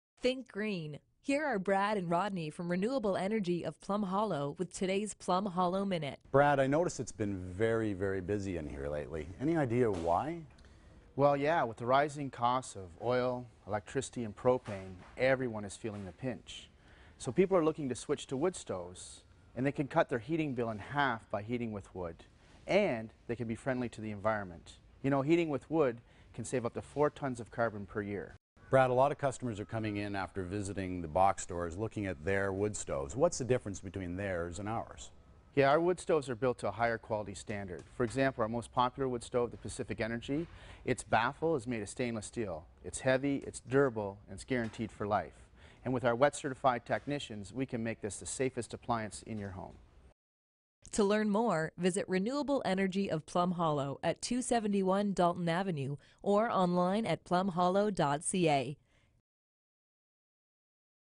Plum Hollow Minute - How converting to a wood stove can save you money
Explaining how with the rising cost of energy switching to a wood stove can cut your energy bills in half as well as being friendly to the environment.
Featuring Pacific Energy.